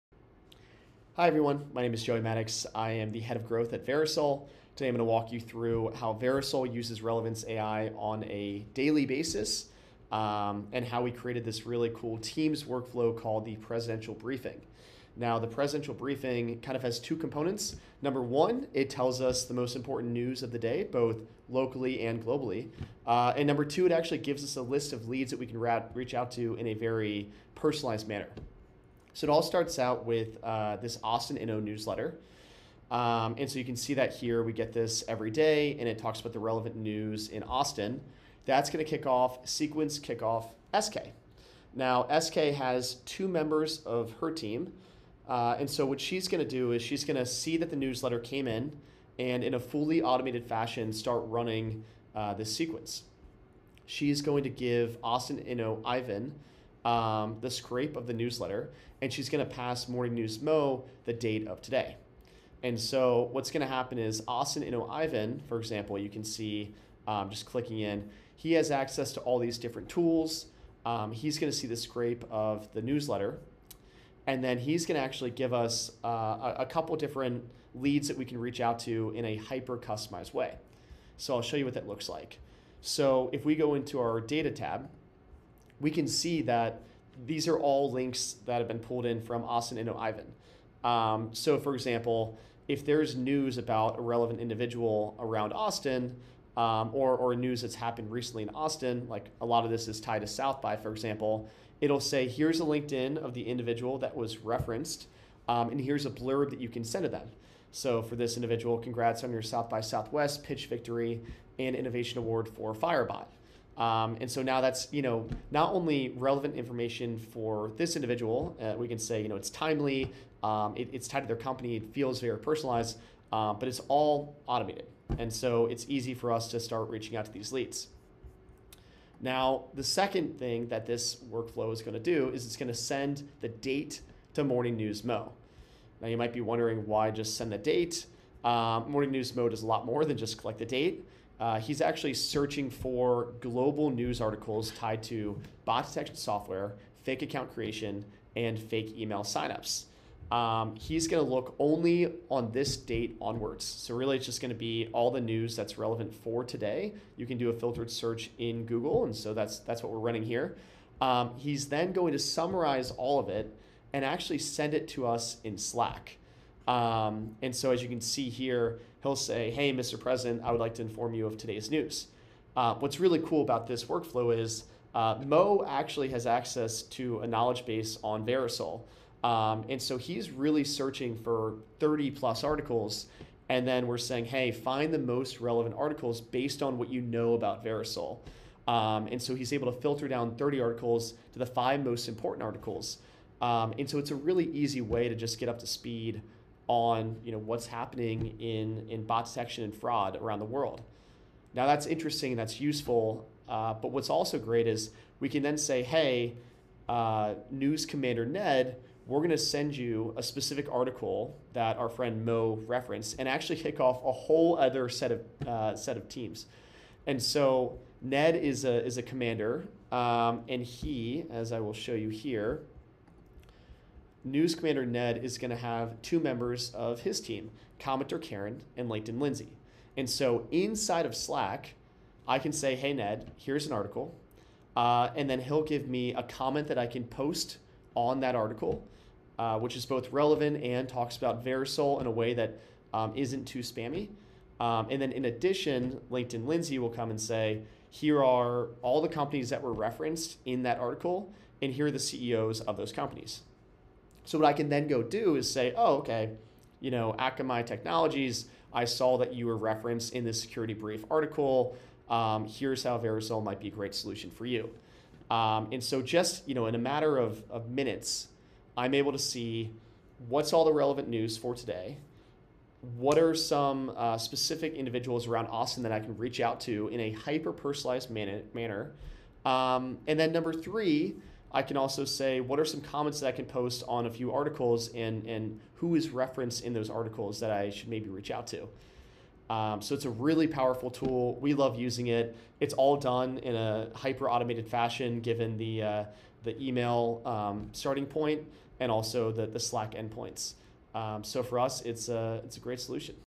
From Tools to Teams: Unlocking Relevance AI’s Full Potential with Verisoul
Learn how the growth team at Verisoul unlocked the full potential of Relevance AI by moving from building simple tools to creating teams of AI agents triggered by automated workflows.